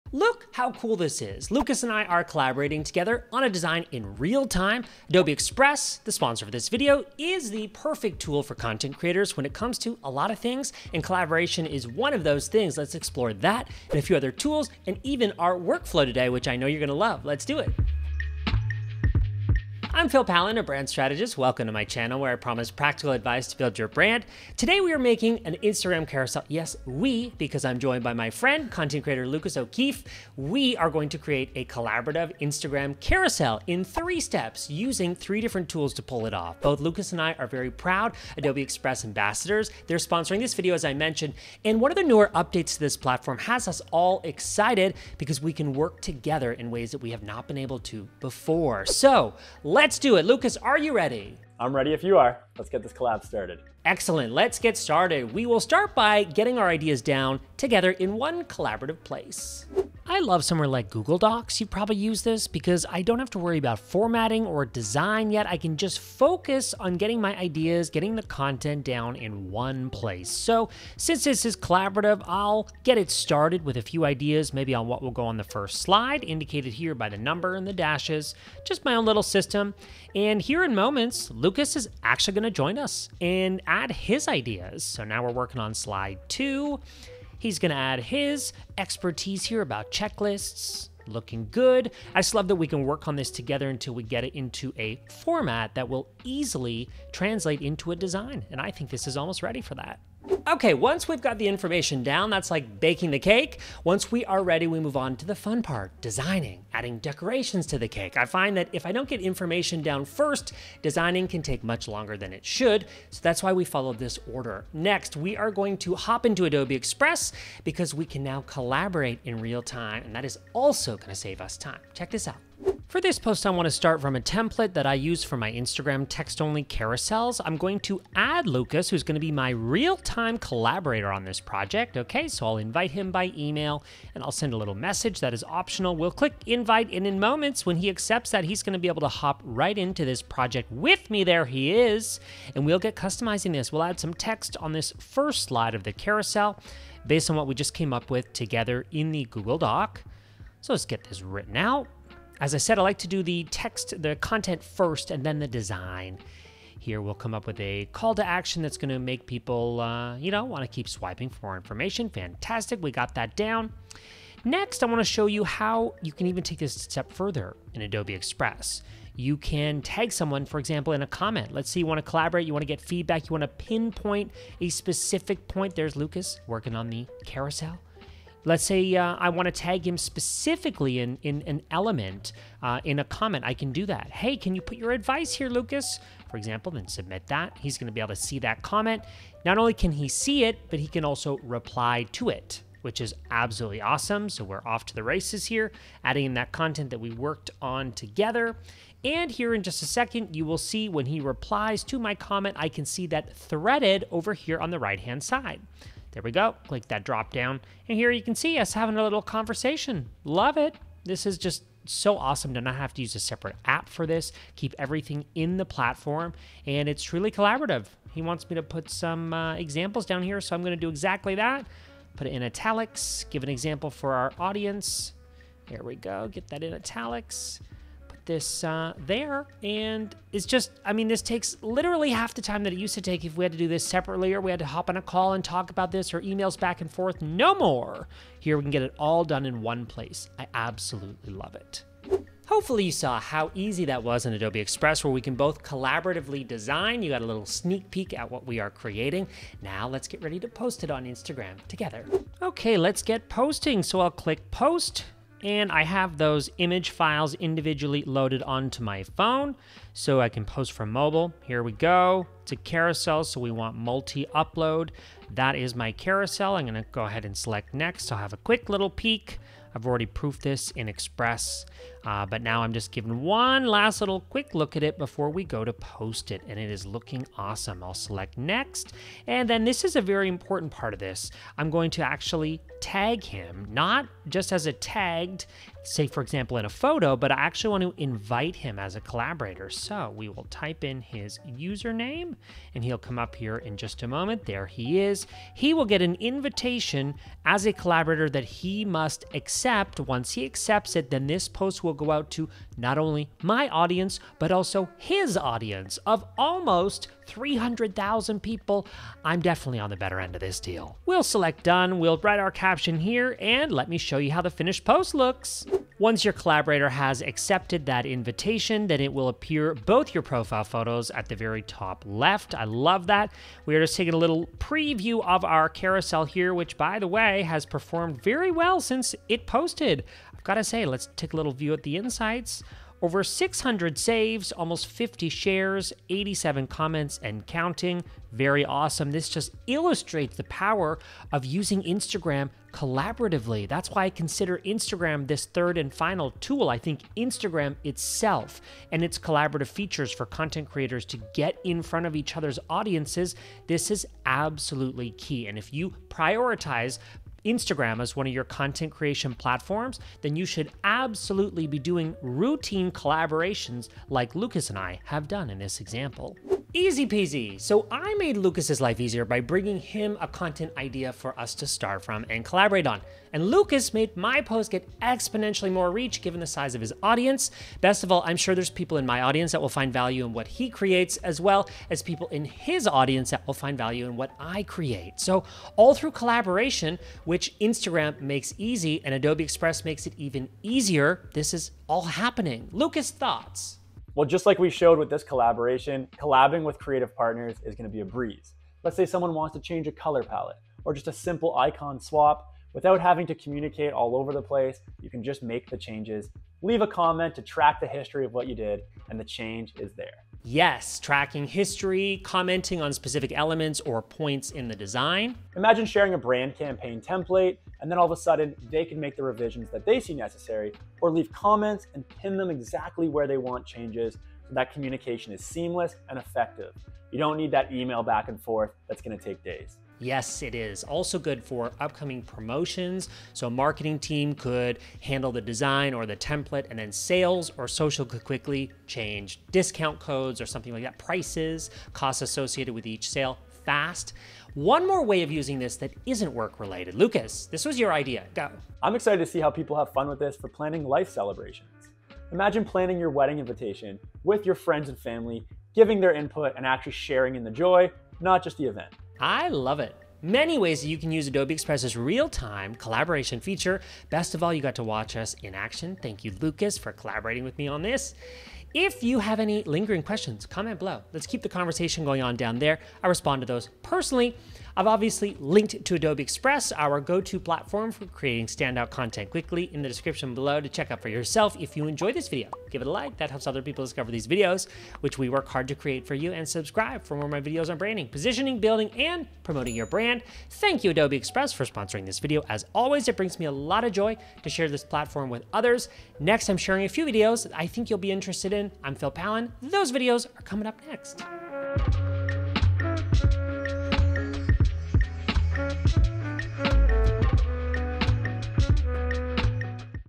Best Real time Collaboration Tools for Content Creators (Adobe Express)
For today's video, I'm joined by content creator Lucas O’Keefe to showcase the power of collaborative design using Adobe Express. We walk you through how to go from concept to completion in three simple steps. It's like baking a cake; first, the idea, then the design! We discuss not just how this tool benefits content creators but anyone who needs collaborative design—from marketing teams to planning life events.

• Real-time collaboration with Adobe Express
• Step-by-step guide on creating an Instagram carousel
• Seamless workflow tips for content creators

Adobe Express, the sponsor of this video, allows us to collaborate in real-time. Experience seamless design workflows and avoid back-and-forths!

👇 QUESTIONS? Drop them in the comments below! I read and respond to each one!

🎬 Chapters:
0:00 Collaboration in Adobe Express
0:49 Getting ideas down together in Google Docs
1:39 Designing the Instagram Carousel in Adobe Express
4:02 Posting the Instagram Carousel on Instagram
6:06 Thoughts on Collaboration
7:38 Conclusion